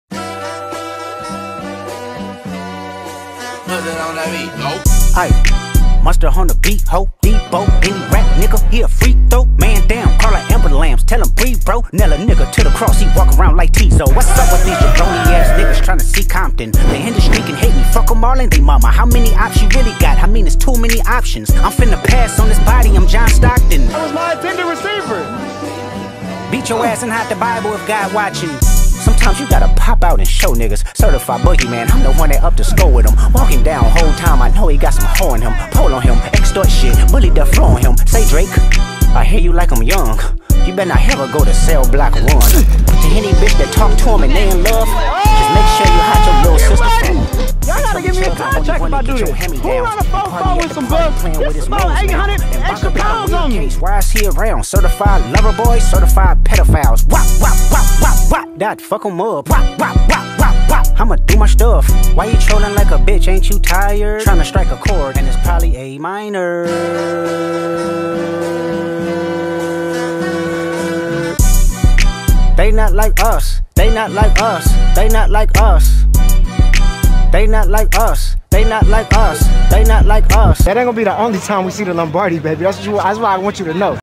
Chef Geech - NFL DRAFT BTS RECAP
Had a Ball at the NFL Draft, my city really showed out!

Shot and Edited by Roger McKenna (@ShootersclubLA)